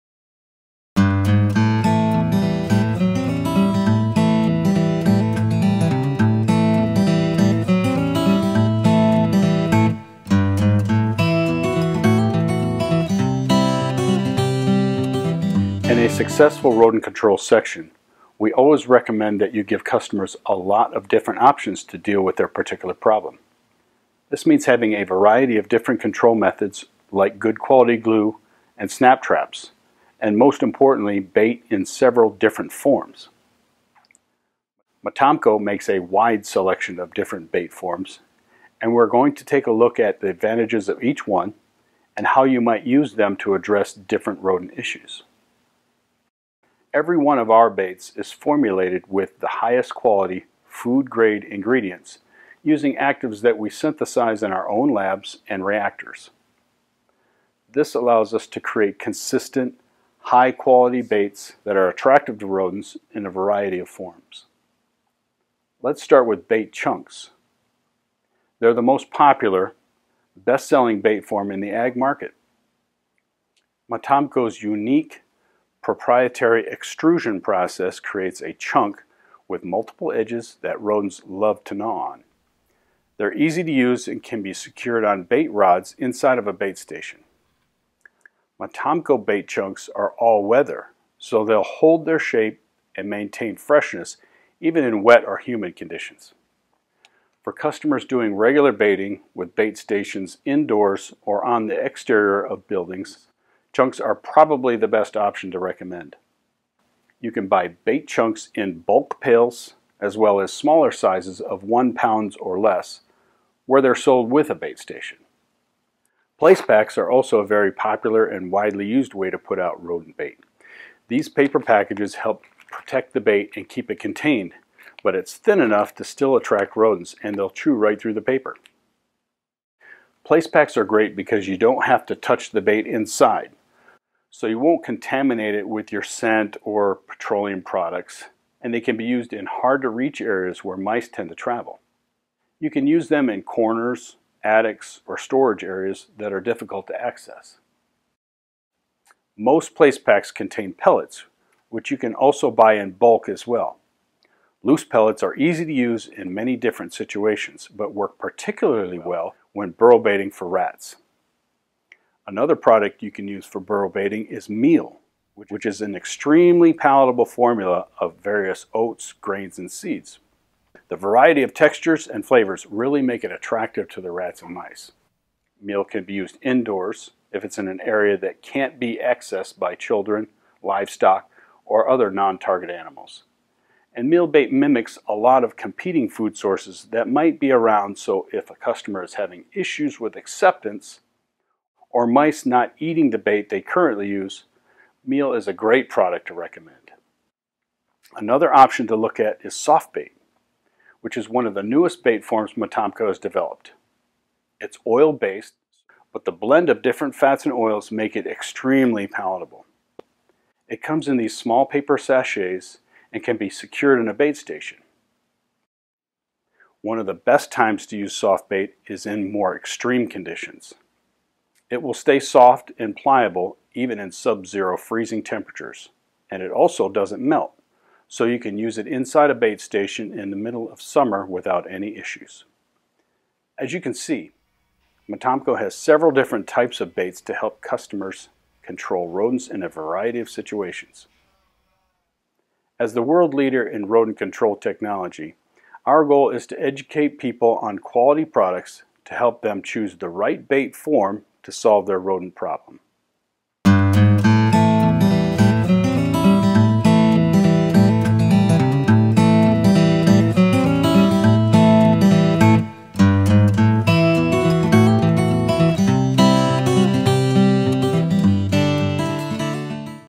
Motomco Rat & Mouse Bait - Bait Forms
Learn about the various Bait Forms available from Motomco and how they can be used to address different rodent control issues